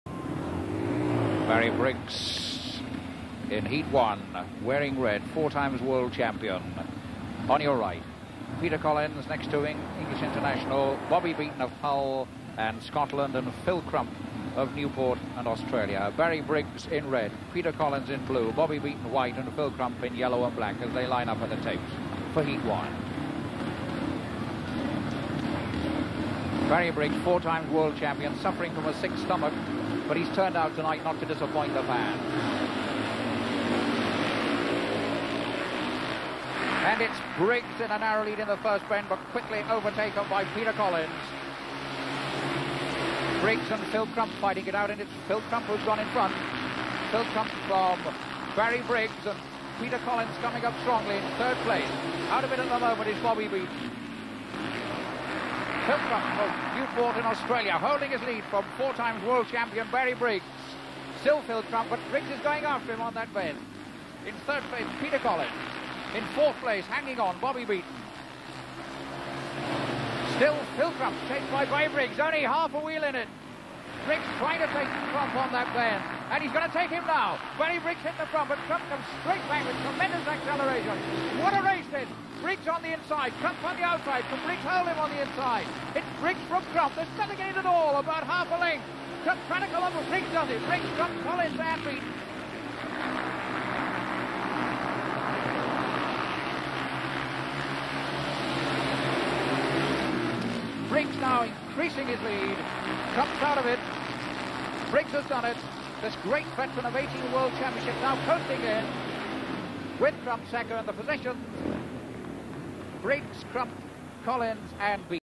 08.21.1974 Yorkshire TV Trophy, The Boulevard, Hull H1 Speedway
Speedway: Yorkshire TV Trophy, The Boulevard, Hull 21st August 1974 Heat 1
Red:  Barry Briggs ,
Blue:  Peter Collins ,
White:  Bobby Beaton ,
Yellow:  Phil Crump ,
ITV Sports